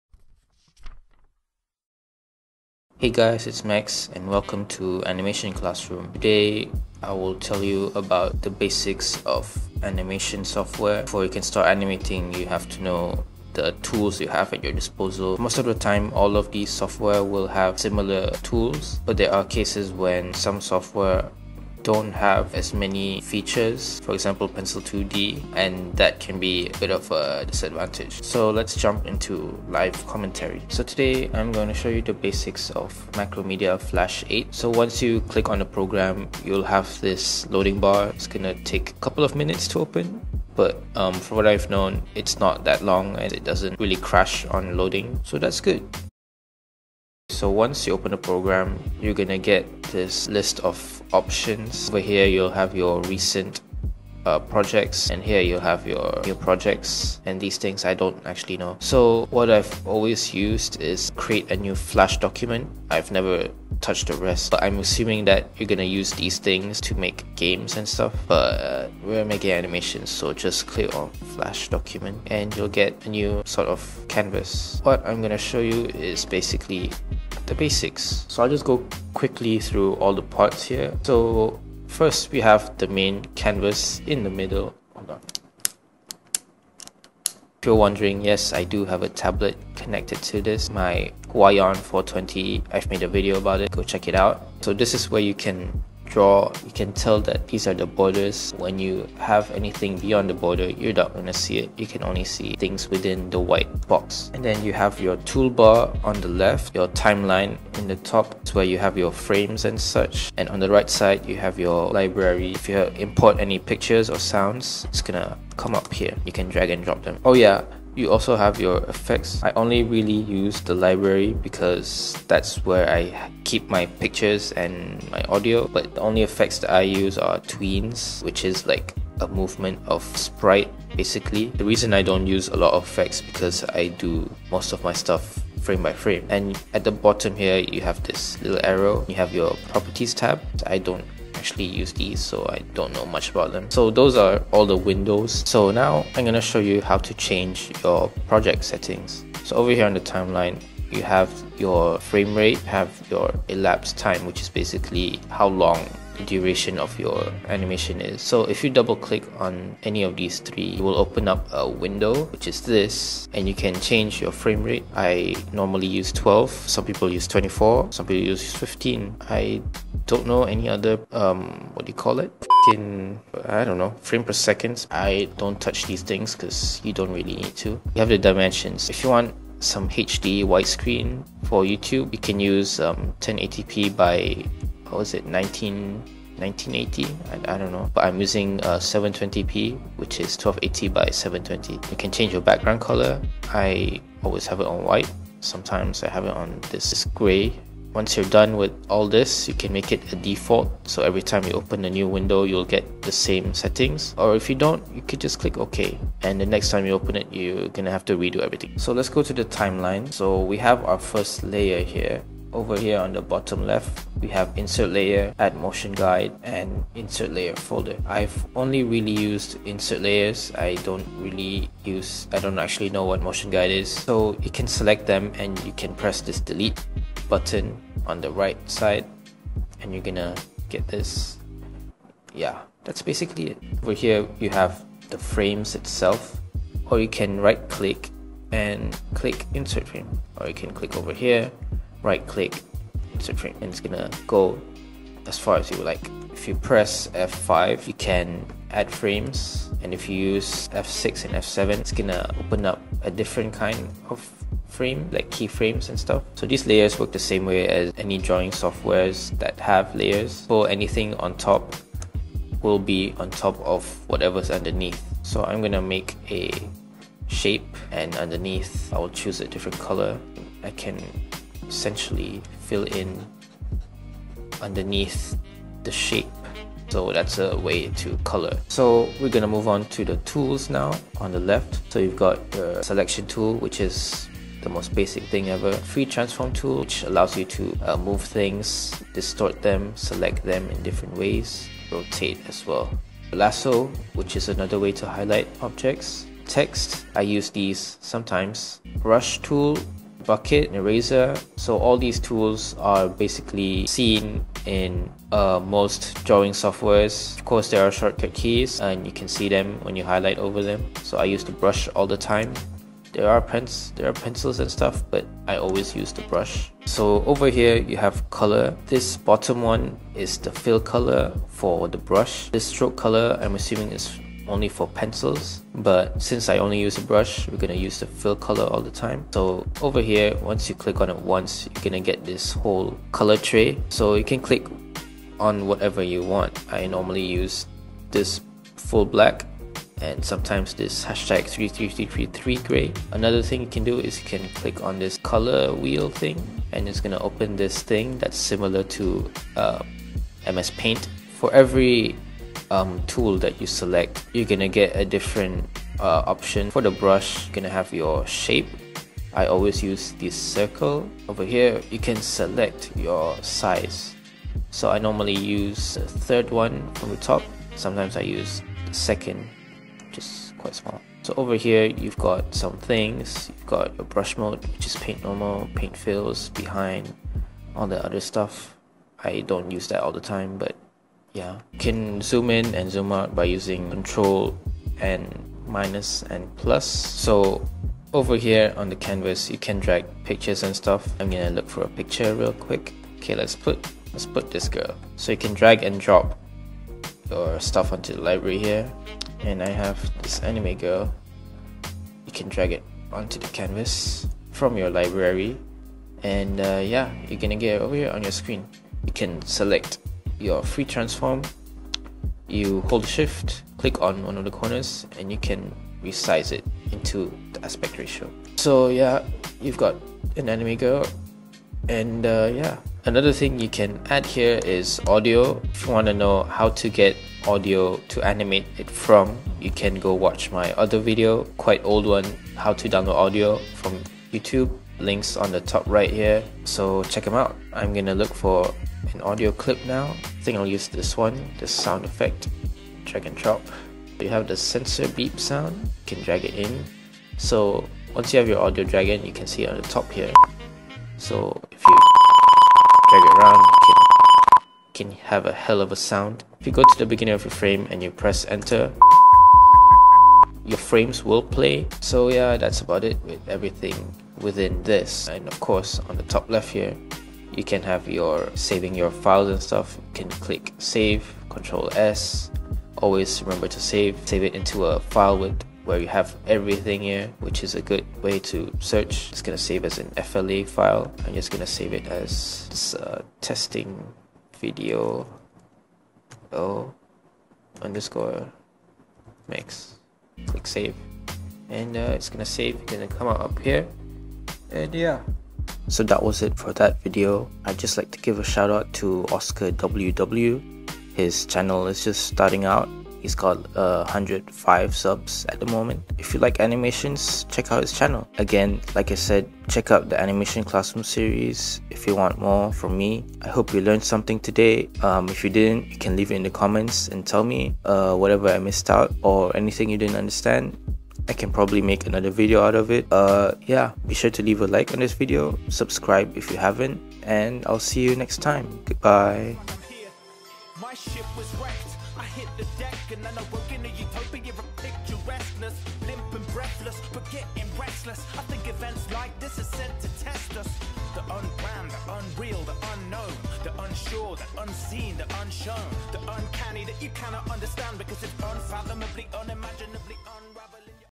How to use Macromedia Flash 8 Tutorial (Animation Classroom)
how to use macromedia flash 8 pro. I try to explain all the features of Macromedia Flash 8 that I use to animate.

Mechs on Instagram: @themultimechs
Mechs on Twitter: @themultimechs